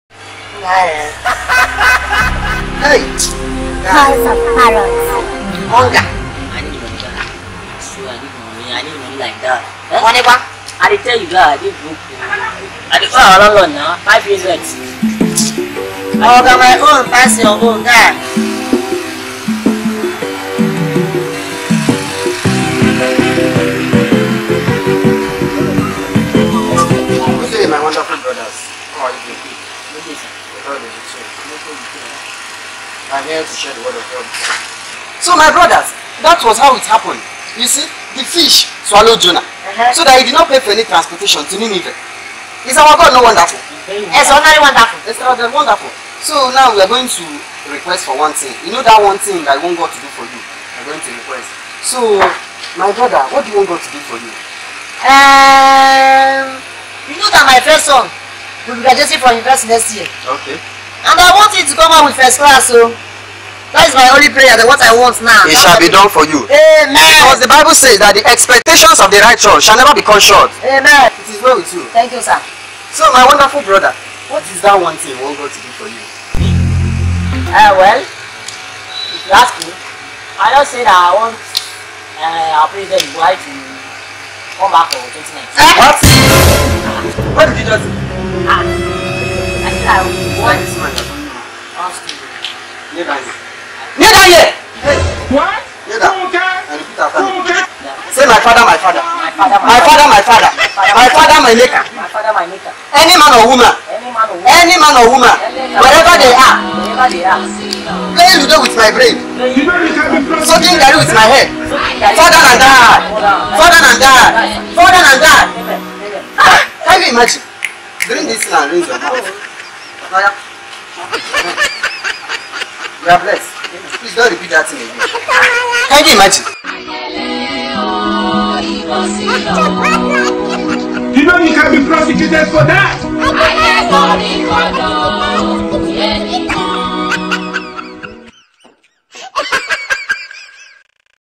WHEN REQUEST GOES WRONG-HOUSE OF PARROT
when a pastor tells you to say your prayer point..and you come dey talk another things..